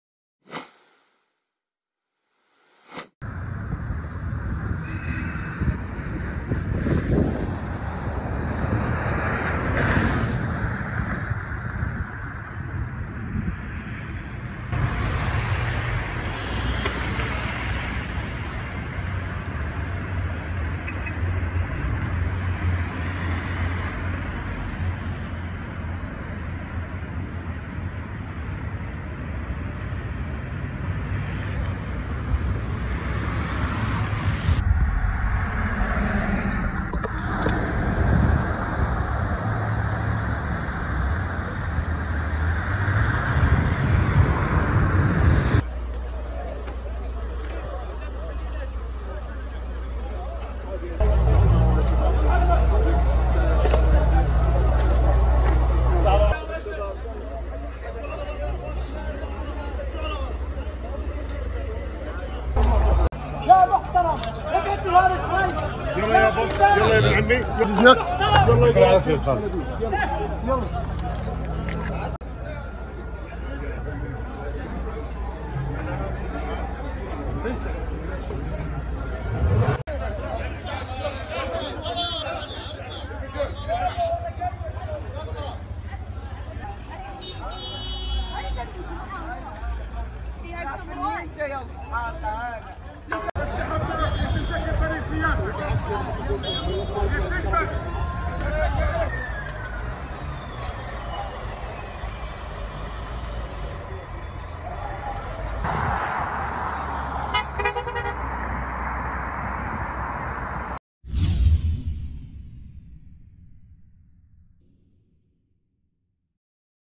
Interceptions light up the sky over Amman amid Iranian attack on Israel
Interceptions light up the sky over Amman amid Iranian attack on Israel. Iran launched an unprecedented drone and missile attack on Israel, the Israeli army announced late Saturday, in a major escalation of the long-running covert war between the regional foes.